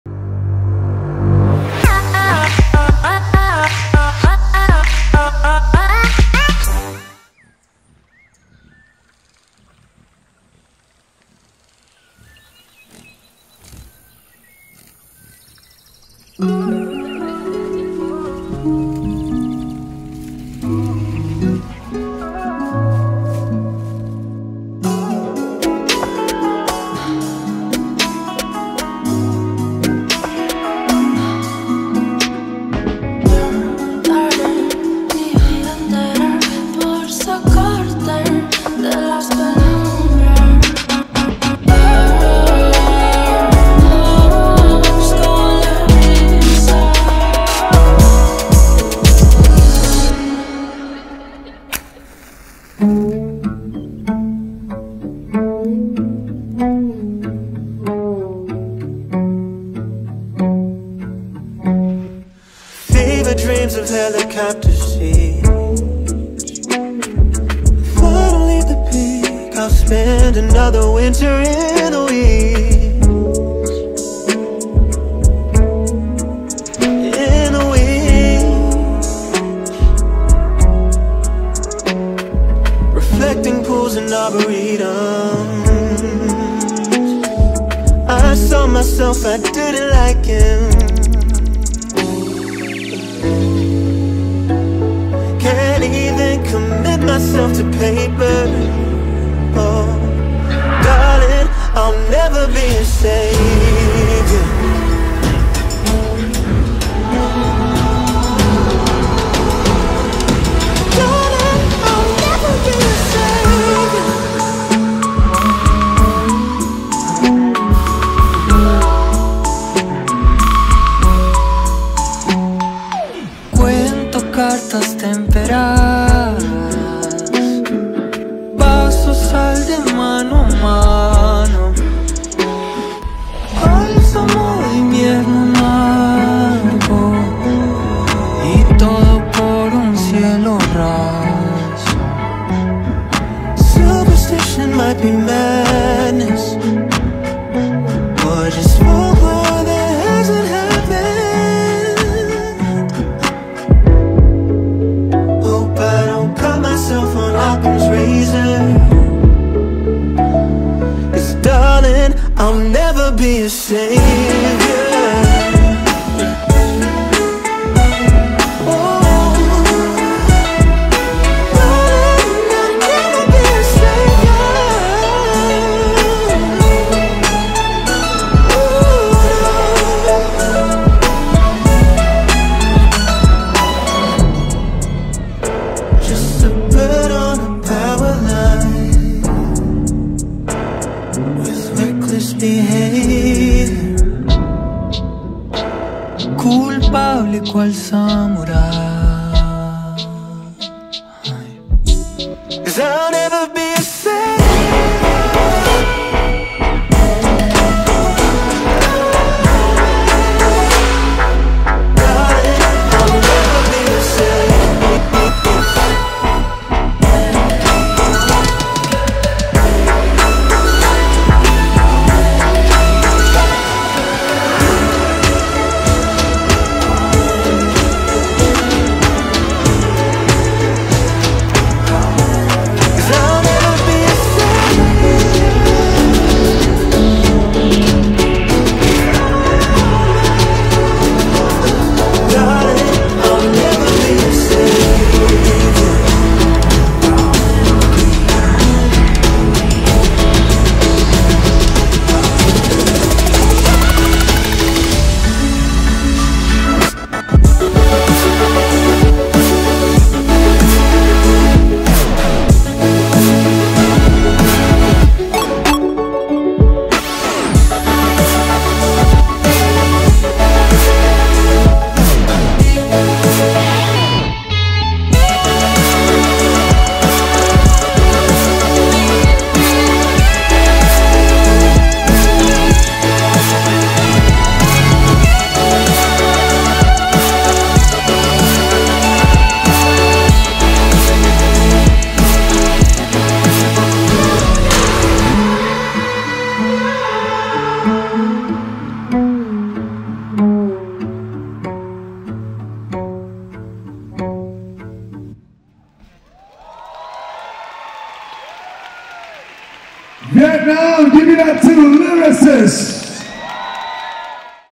LYRICÍST | Summer Jam Showcase Competition 2023 [@VIBRVNCY Front Row 4K]
LYRICÍST
Media Coverage by @VIBRVNCY
Filmed by Chad Mayate @chad.mayate
SUMMER JAM Showcase Competition presented by Recognize! Studios & Jam Republic
CENTER EXPO Da Nang, Vietnam